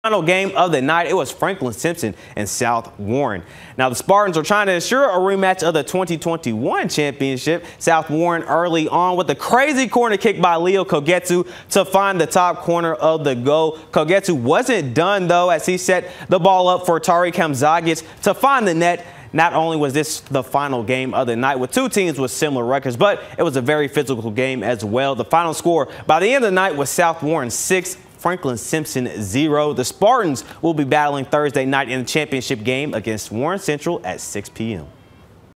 4th Region Boys Soccer semifinals: Franklin Simpson vs. South Warren
Spartans beat Wildcats 6-0